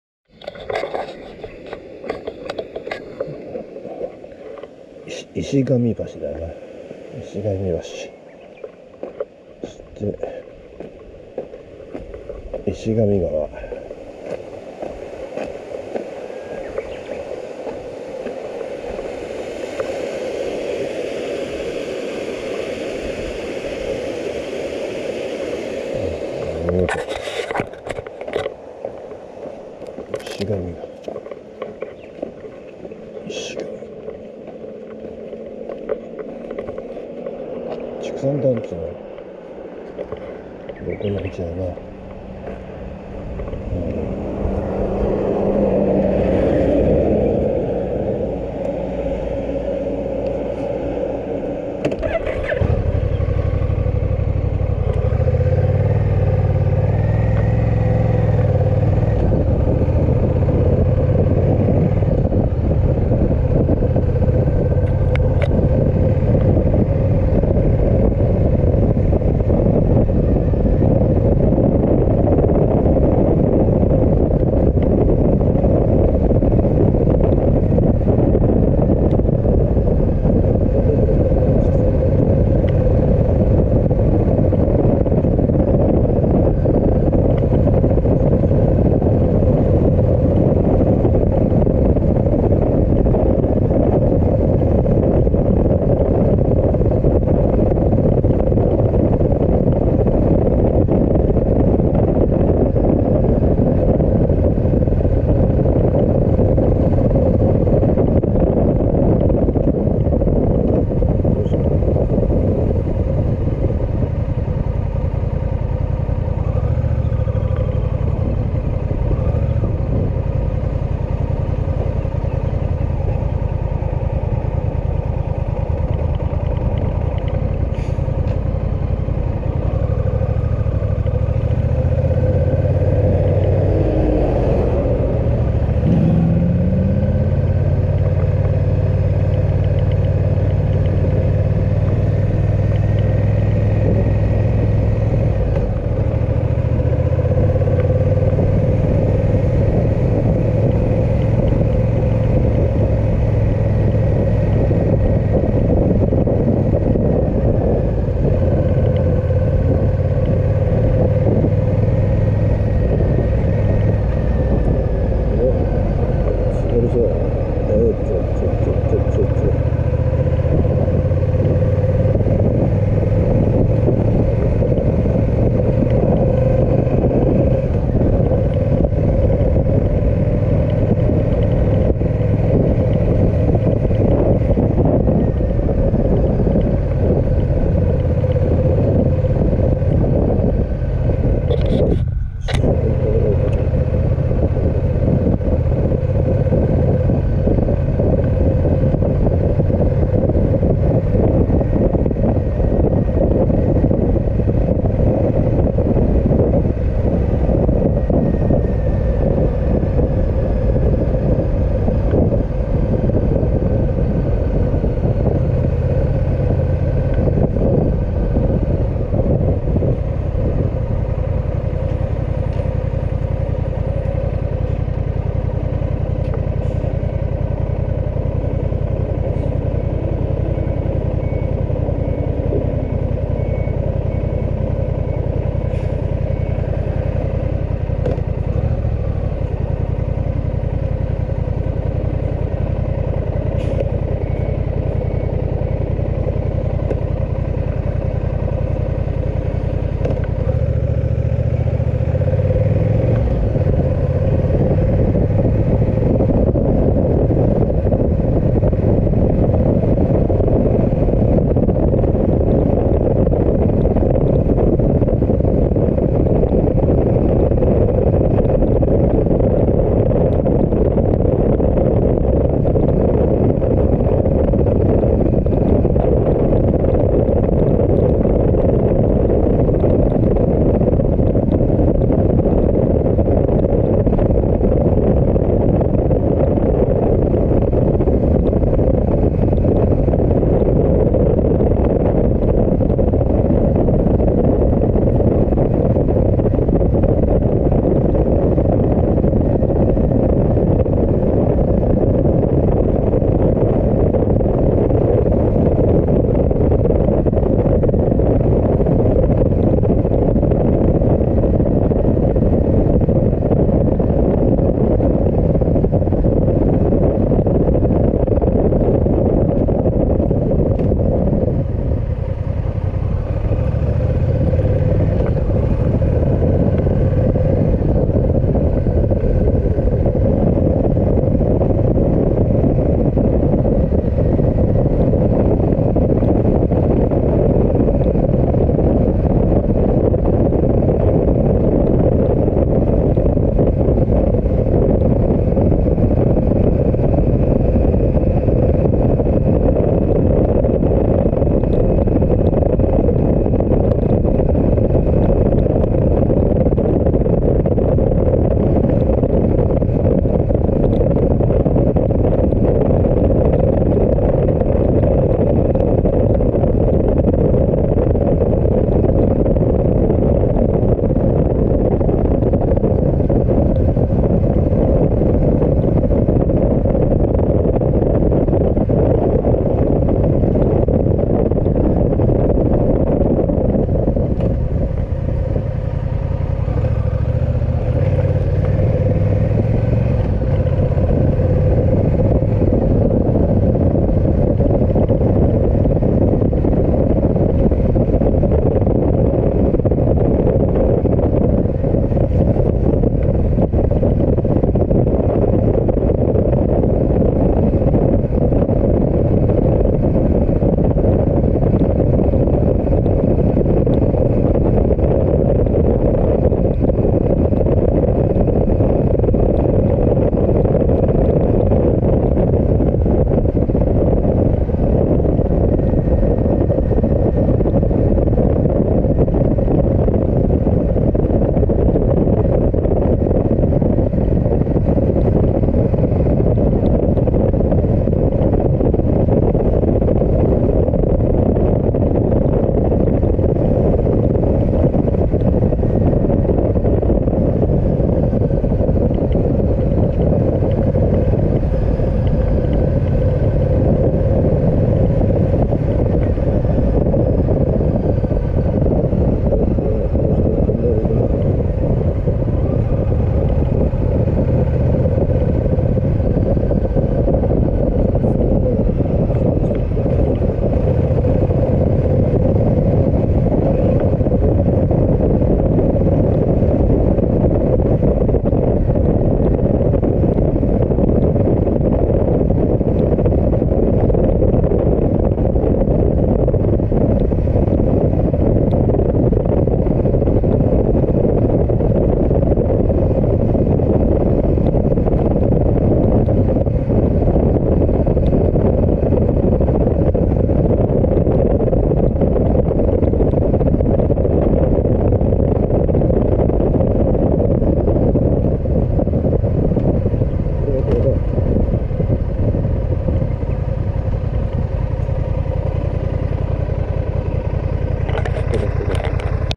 石神橋から GX010069
通行止めだった石神川の石神橋から中野方面